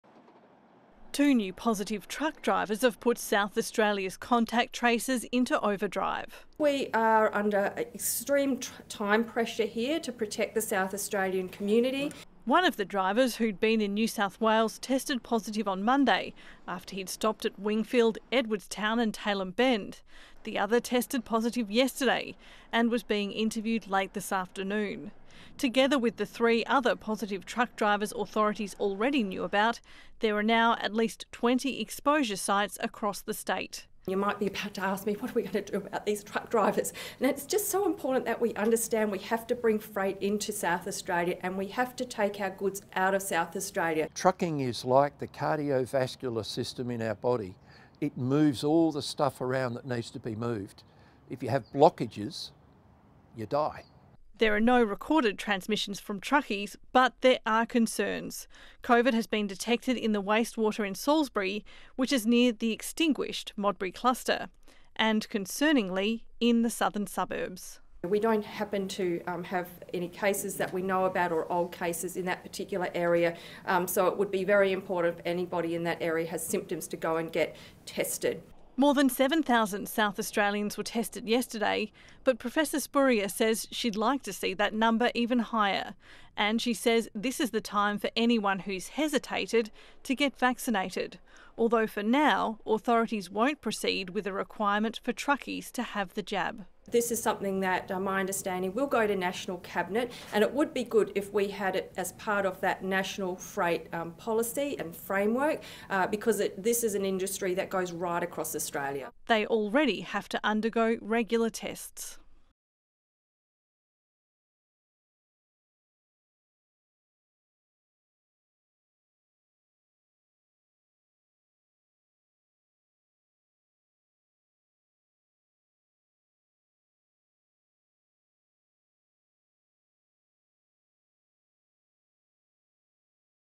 Interstate truck drivers test positive for COVID-19 in SA as virus detected in wastewater | ABC News
South Australian health authorities have revealed that five COVID-positive truck drivers have travelled through the state in the past six days, while new wastewater testing results have revealed traces of the virus in Adelaide's north and south.